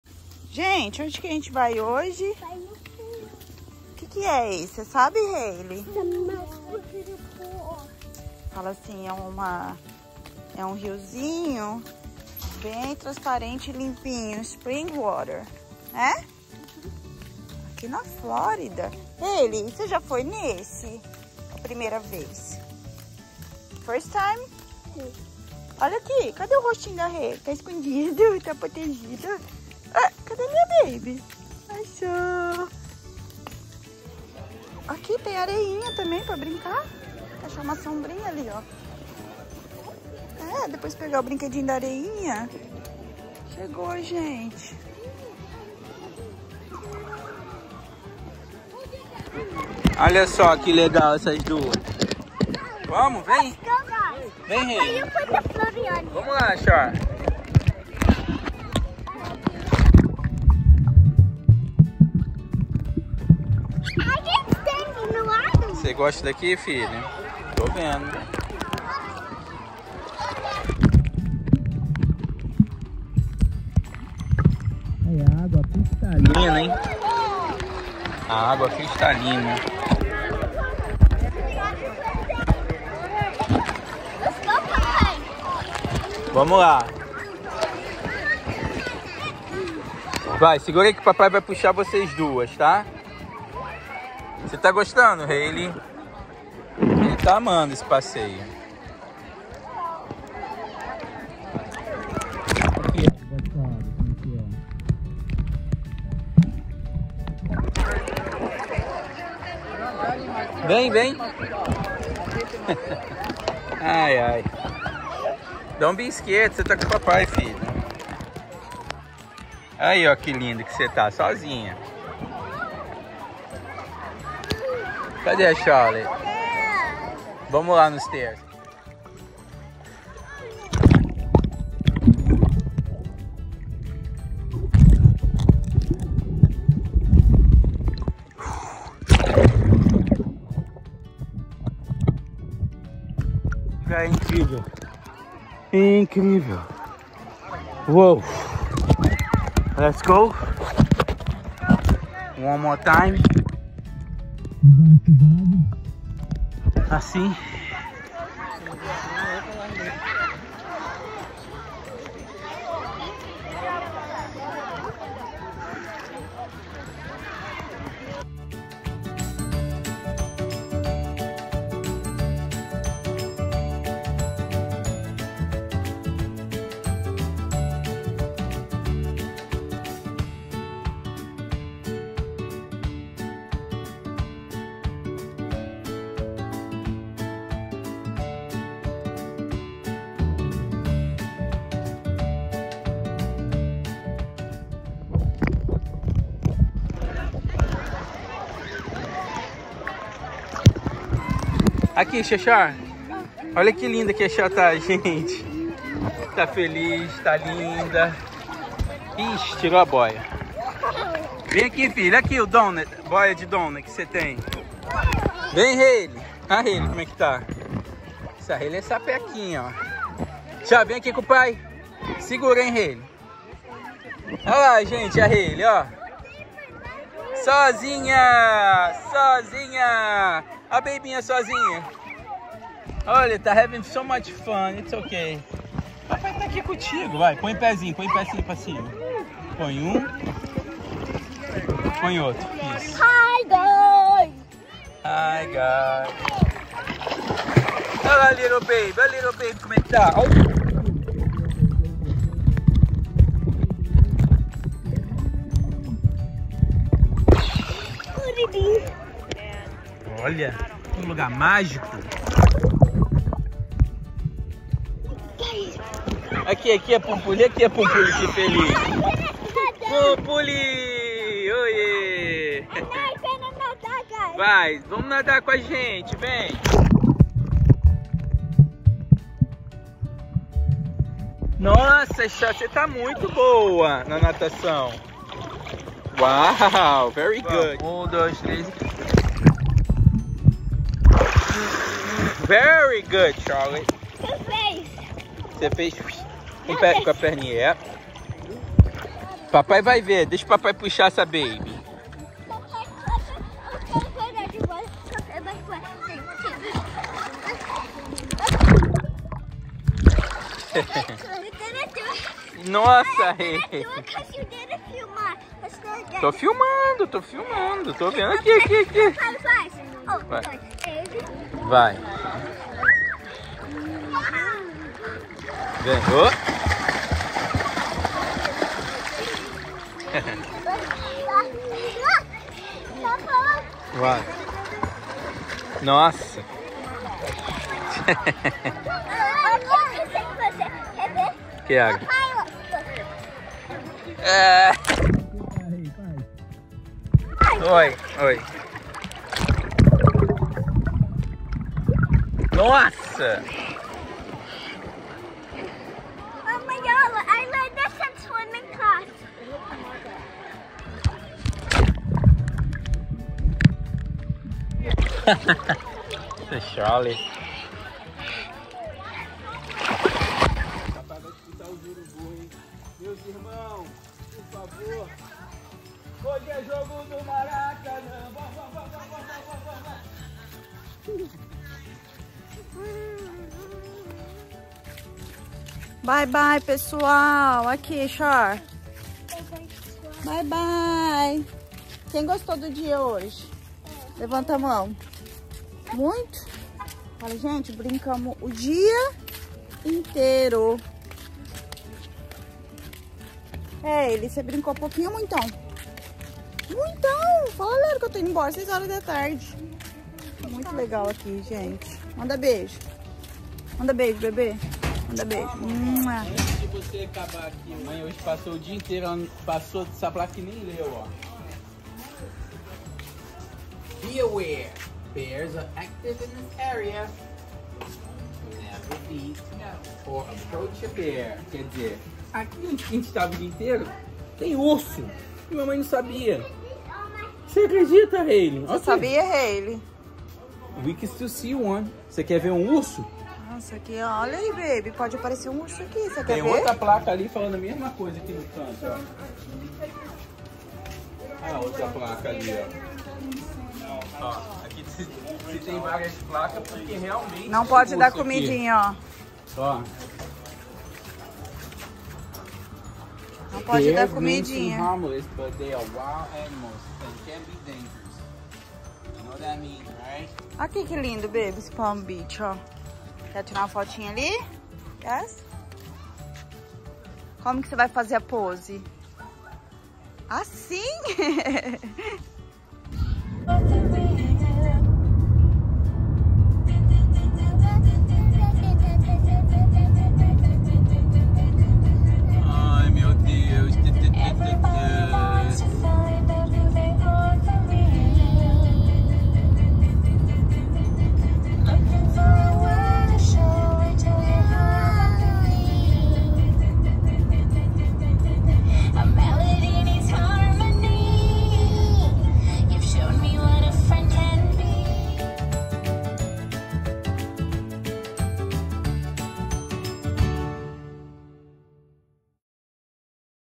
UM DIA INTEIRO NAS PISCINAS NATURAIS DA FLÓRIDA | Alexander Springs
Ola seja bem vindo ao nosso canal 🤗 Charlotte e Hailee sao irmãs nascidas nos EUA e de pais brasileiros. Aqui compartilhamos muita diversão, brincadeiras e nossos passeios em família.